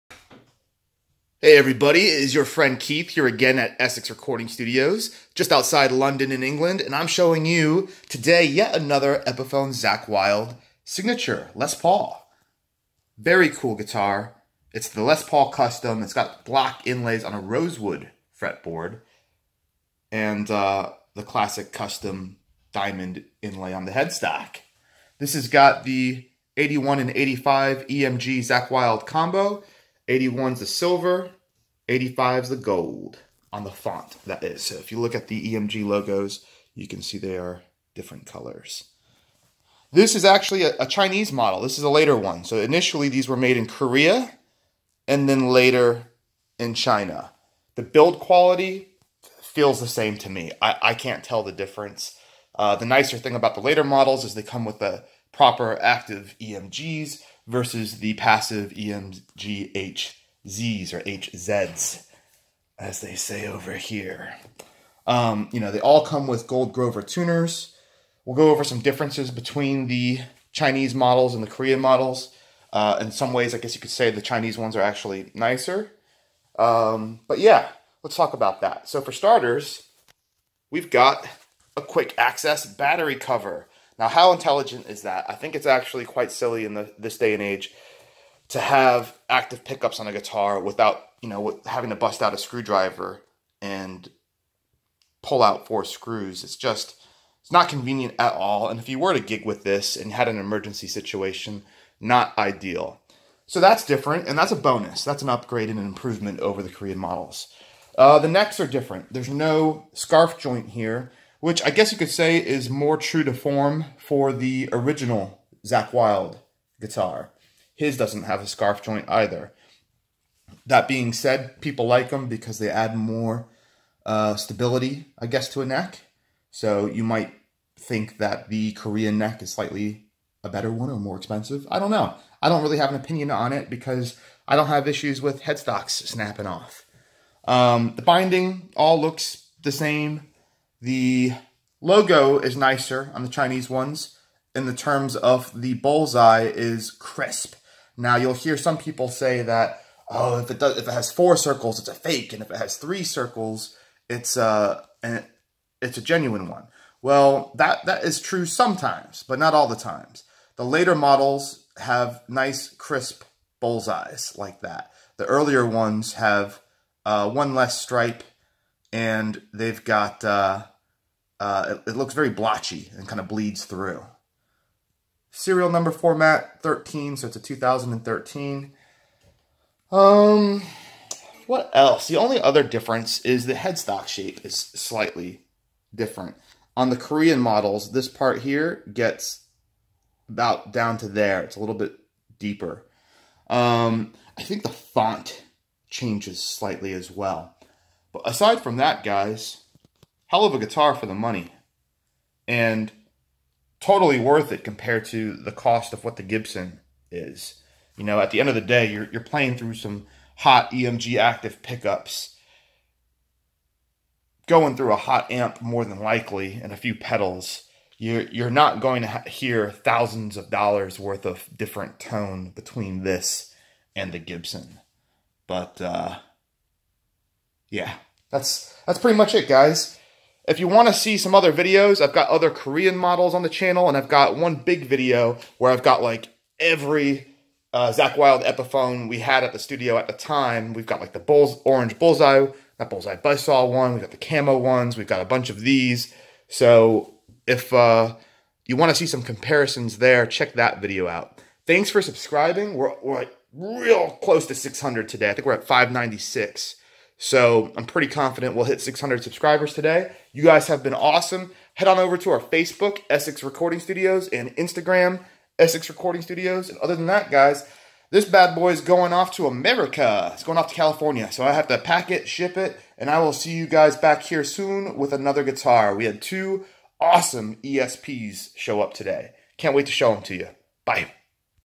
2013 EPIPHONE CHINESE ZAKK WYLDE LES PAUL CUSTOM BULLSEYE MADE IN CHINA EMG 81 85 UP CLOSE VIDEO
2013 EPIPHONE CHINESE ZAKK WYLDE LES PAUL CUSTOM BULLSEYE MADE IN CHINA EMG 81 85 UP CLOSE VIDEO